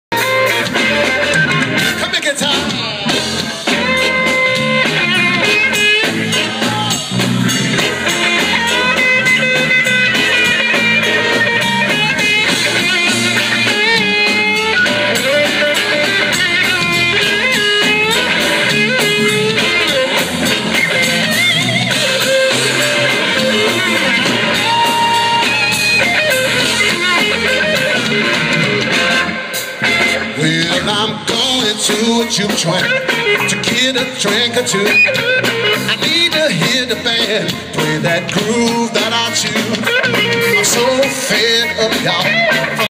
Ronnie Baker Brooks Shreds: Guitar Solo at Westport CT Blues Fest 9/06/15
Ronnie Baker Brooks Guitar Solo @ Westport CT Blues Festival 9/06/15. Amazing Chicago bluesman in blues, funk and soul.